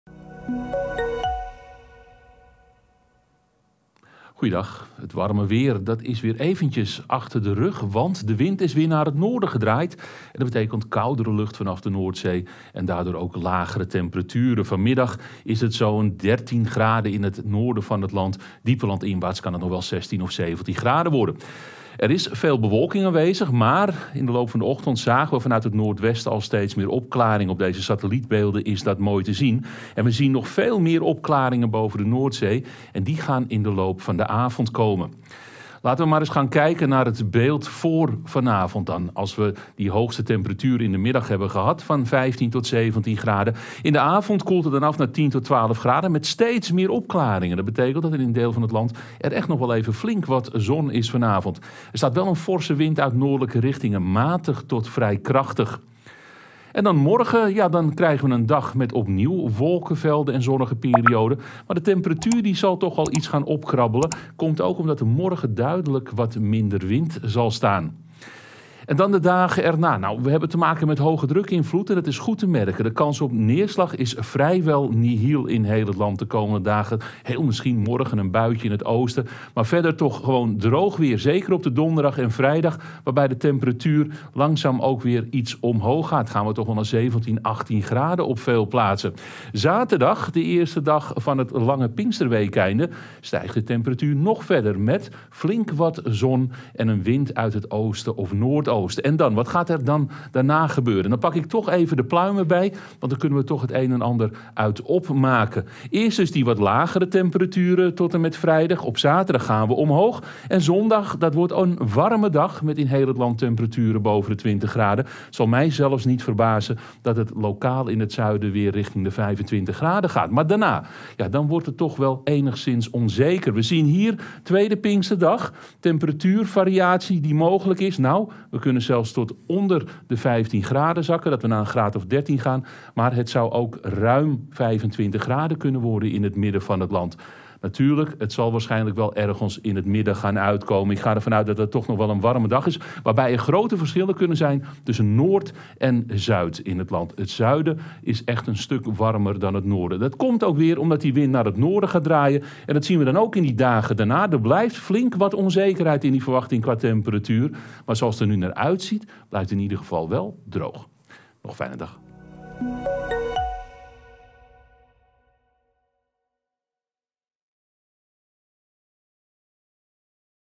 Weerbericht: geleidelijk meer zon en warmer
De noordelijke wind maakt het op veel plaatsen fris. De komende dagen gaat de temperatuur geleidelijk weer omhoog.